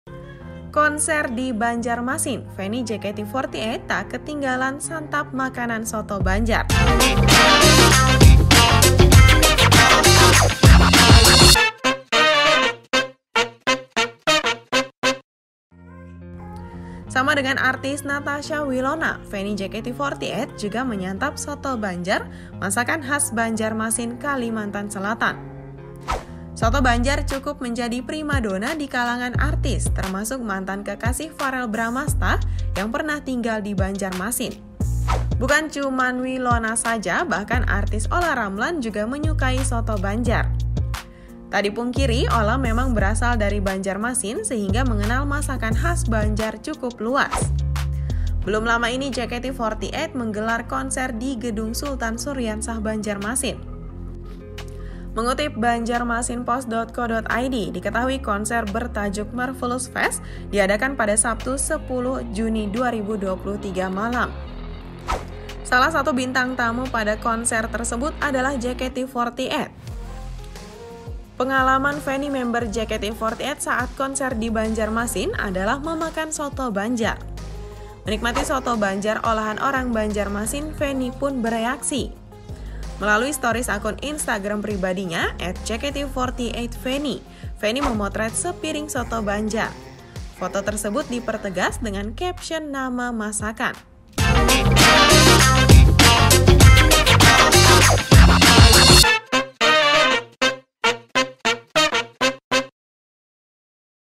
Seperti Natasha Wilona, Ini Reaksi Feni JKT48 Santap Soto Banjar Kala Konser di Banjarmasin
BANJARMASINPOST.CO.ID - Sama dengan aktris Natasha Wilona, Feni JKT48 juga menyantap soto Banjar masakan khas Banjarmasin Kalimantan Selatan (Kalsel).

Soto Banjar cukup menjadi primadona di kalangan artis, termasuk mantan kekasih Verrell Bramasta yang pernah tinggal di Banjarmasin.

Bukan cuma Wilona saja, bahkan artis Olla Ramlan juga menyukai soto Banjar itu.